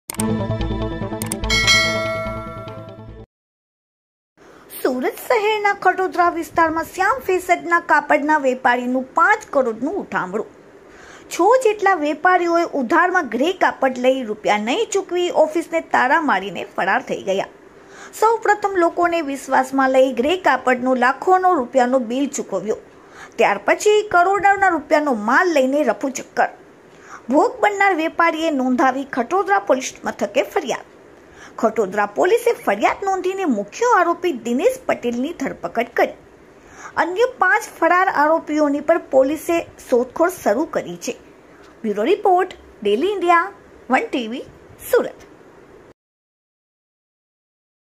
સુરતના ખોટોદરામા શ્યામ ફેશનના કાપડના વેપારીનુ પાંચ કરોડનુ ઉઠામણુ.ખોટોદરા પોલીસે મુખ્ય આરોપીની ધરપકડ
સુરતમાં ખોટોદરા વિસ્તારમાં શ્યામ ફેશન ના કાપડના વેપારીનું પાંચ કરોડનું ઉઠામણું. ગ્રે કાપડ ના પૈસા નહીં ચૂકવી ઓફિસને તાળાં મારી ફરાર.ખોટોદરા પોલીસે એક મુખ્ય આરોપીની ધરપકડ કરી.dailyindiatv dailyindia1tv surat suratnews dailyindia media surati zarareporter press suratsamachar ‎@Daily India Tv  dailyindiatvchannel

અમારા ચેનલ ને લાઈક શેર અને સબસ્ક્રાઇબ કરો.